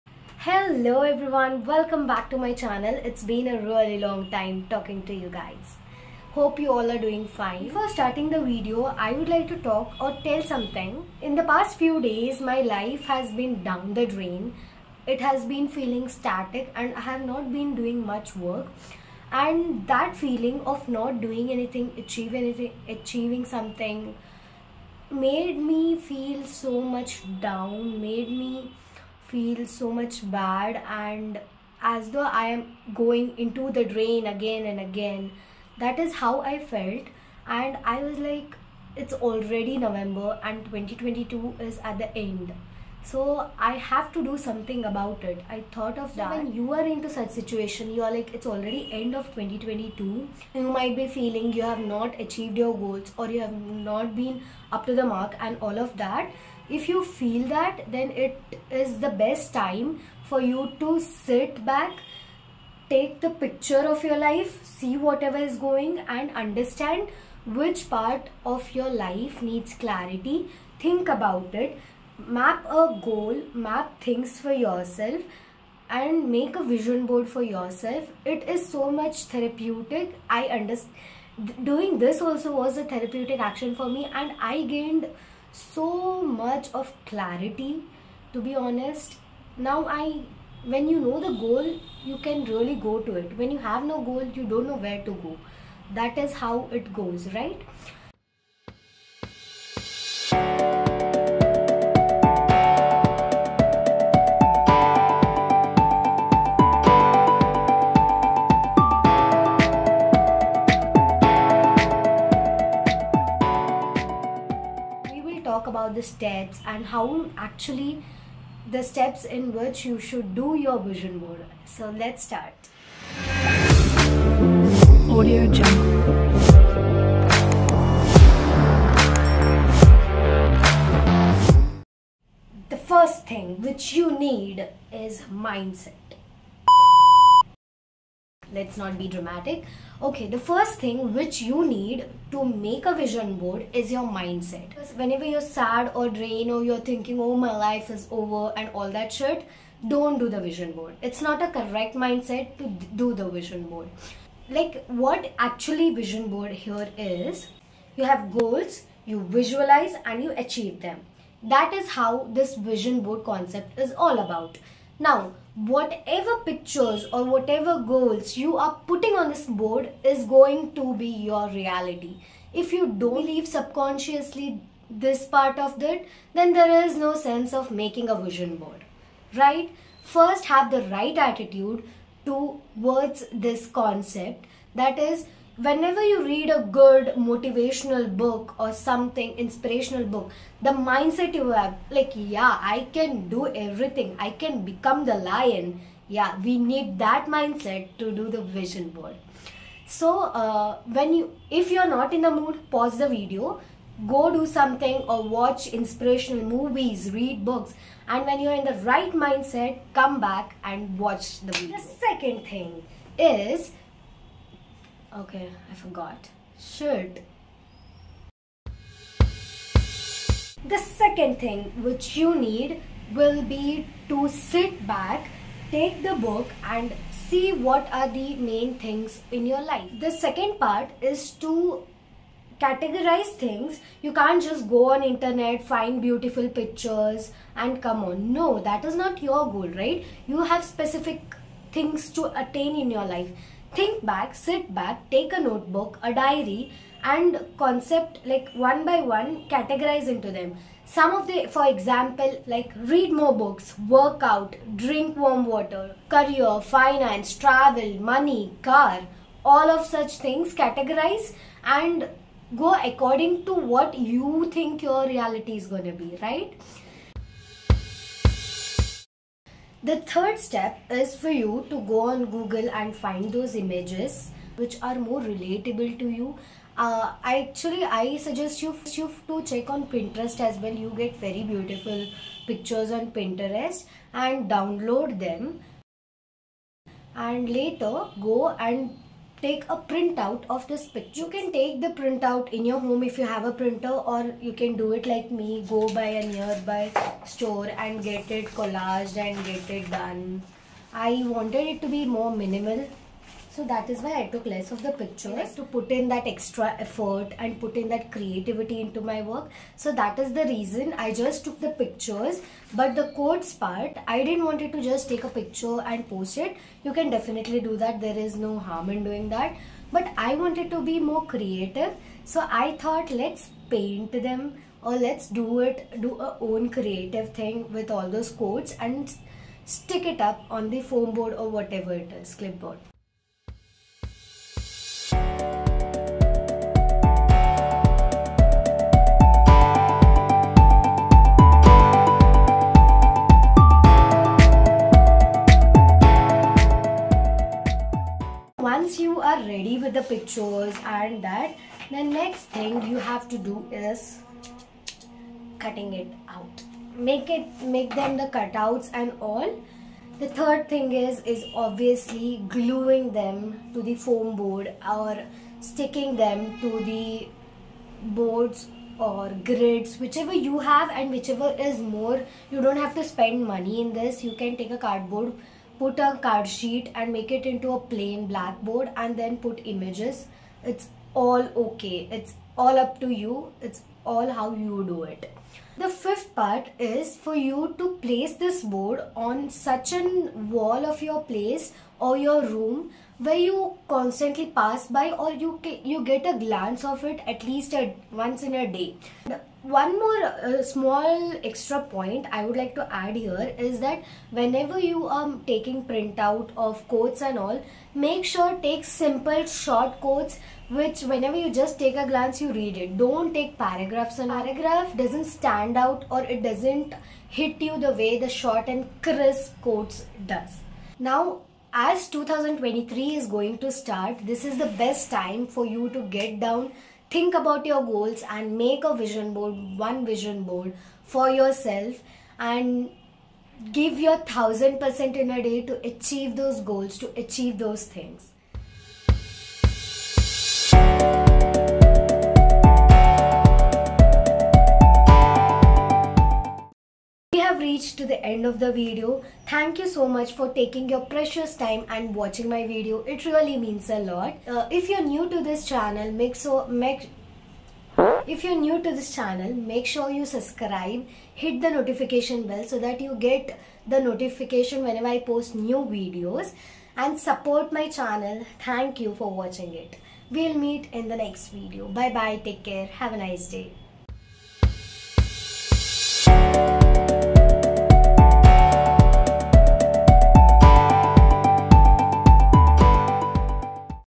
VISION BOARD 2023 | How to make vision board in 2023?
In this video you will get tips to create your own vision board in the brand new 2023
How to make a vision board that will help you achieve your goals in the year 2023!  This video is all about vision board , Law of attraction , Manifestation and a step by step guide to make a vision board for yourself.

Are you looking for a way to help you achieve your goals in the year 2023? If so, then a vision board may be the perfect solution for you!

In this video, I'll show you how to make a vision board that will help you achieve your goals in the year 2023.vision boards are a great way to stay motivated and focused, and can help you stay on track with your goals. By creating a vision board, you'll be able to see your goals more clearly and make progress towards them!

Vision board :
0:00  : Intro
1:32  : Things needed to make Vision Board
2:12 : Step 1: Mindset
3:33  : Step 2 : categorize
4:28  :  Step 3 : Google
5:53  : Step 4 : cut and paste
6:29  : Step 5 :  put on appropriate  wall

Hello, I'm  Dr. Aishwarya Hosmath

I've tried my best to deliver this content